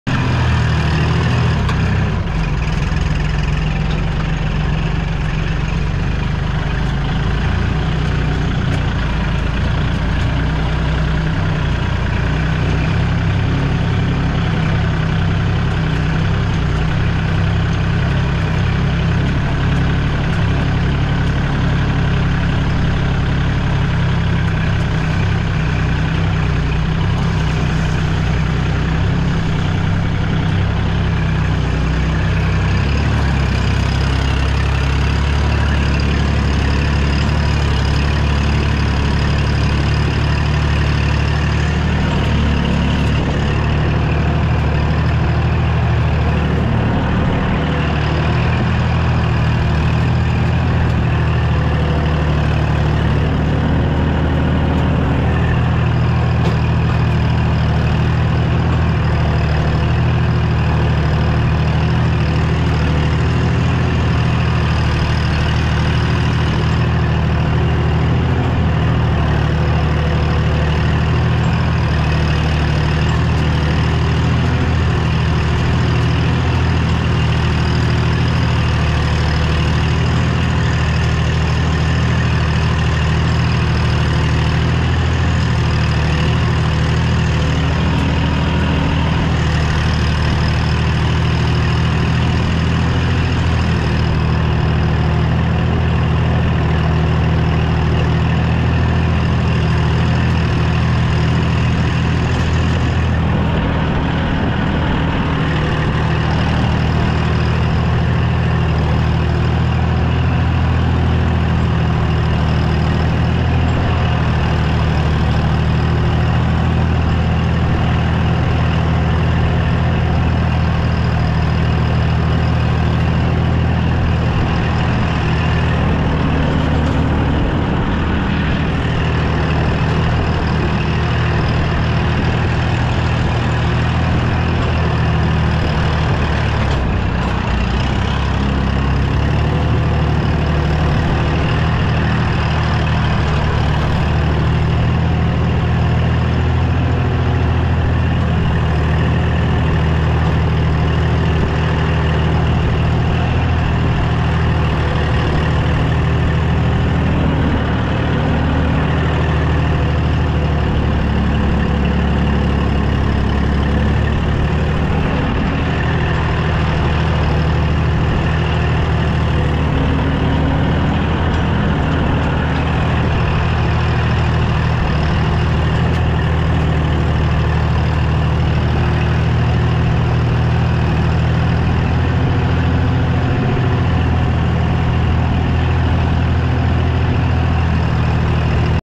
ผานทำรุ่นมัน กำจัดหญ้า ร่องมัน รถไถ ล้อสูง คูโบต้า L4018 สนใจ ล้อ ผาน 080-4802743 ทีมงาน คนป่าทำไร่
ถ้าชอบอย่าลืมกดไลค์และกดติดตามเพื่อรับชมคลิปใหม่ๆเกี่ยวกับ เครื่องจักรเกษตร ด้วยนะครับ
เฟสบุค วิถีเกษตร คนเอามัน